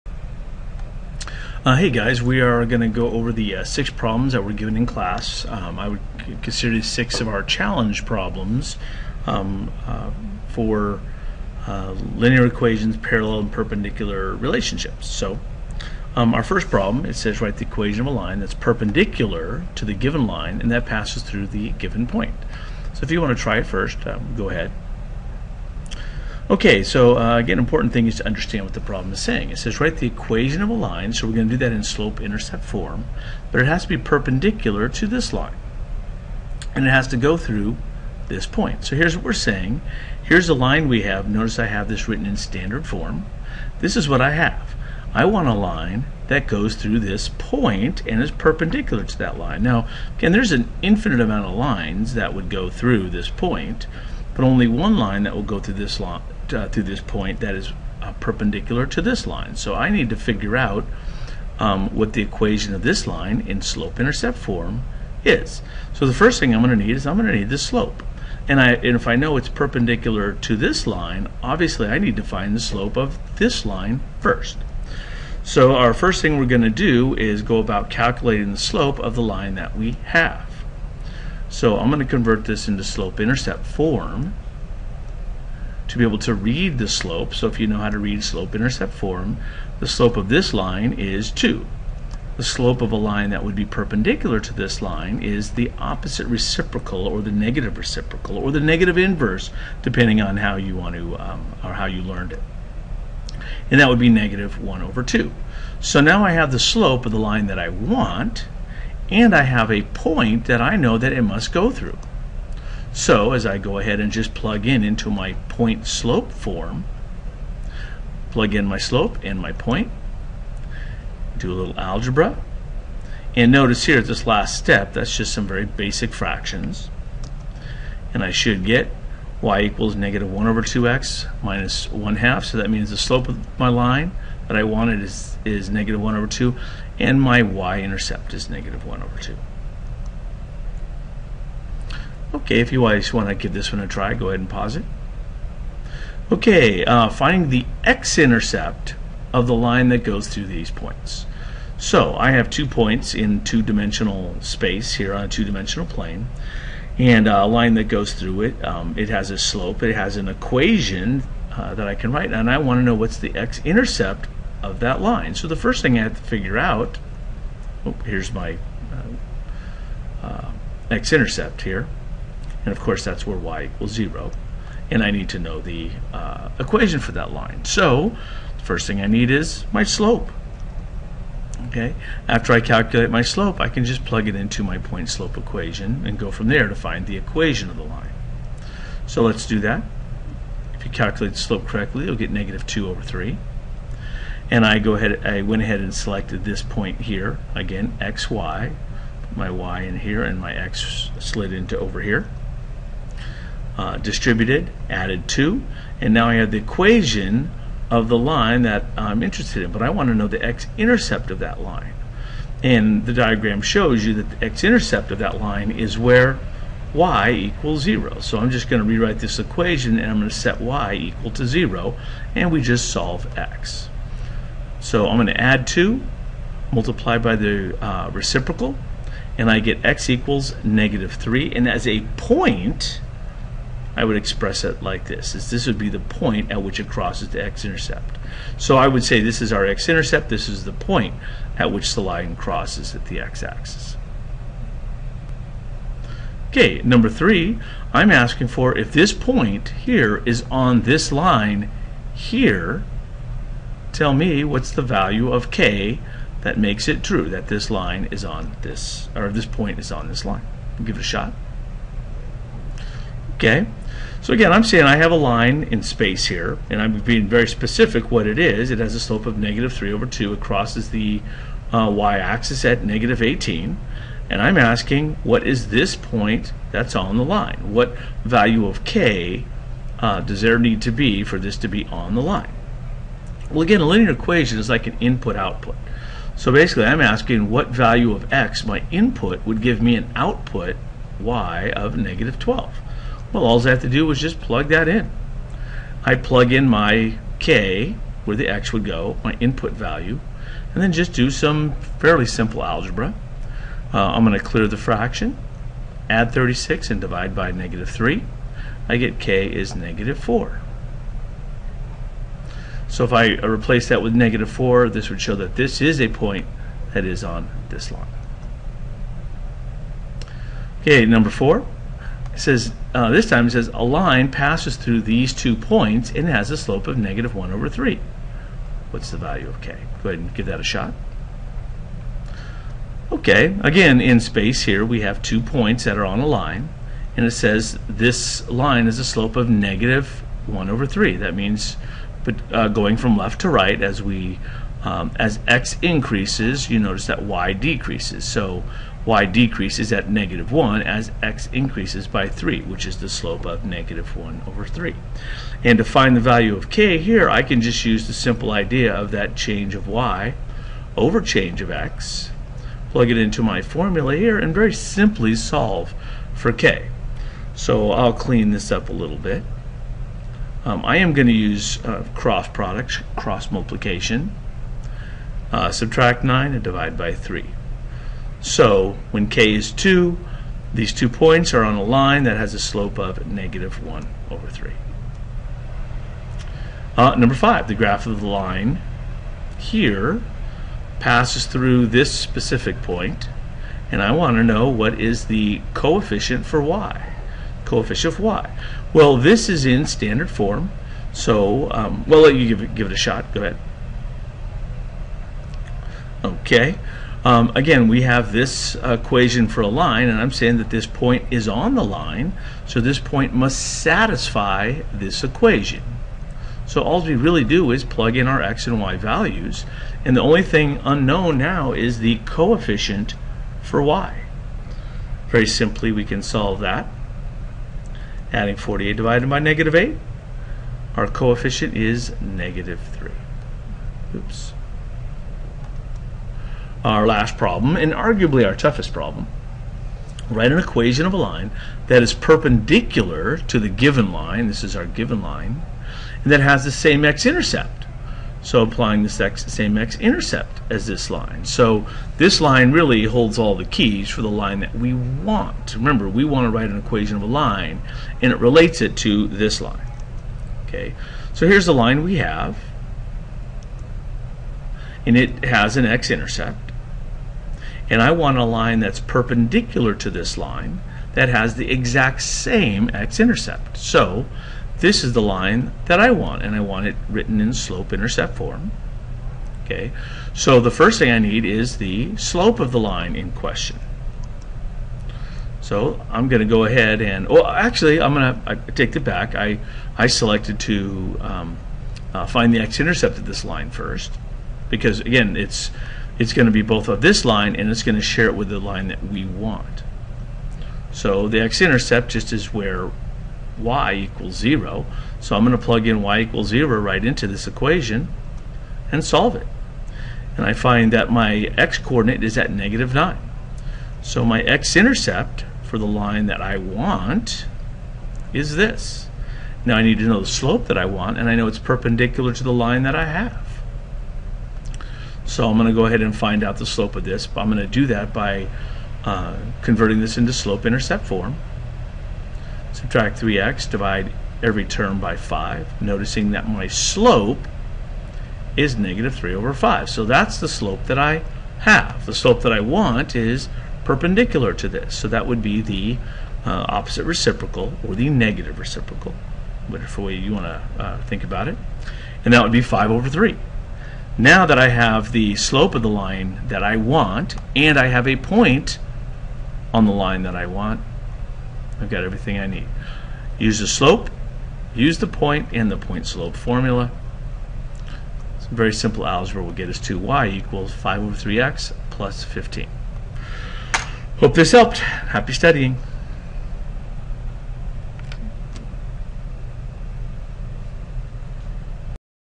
Linear Equation Challenge Problems
These are 6 problems that our class did for classwork that requires many of the skills learned during our time working with linear equations.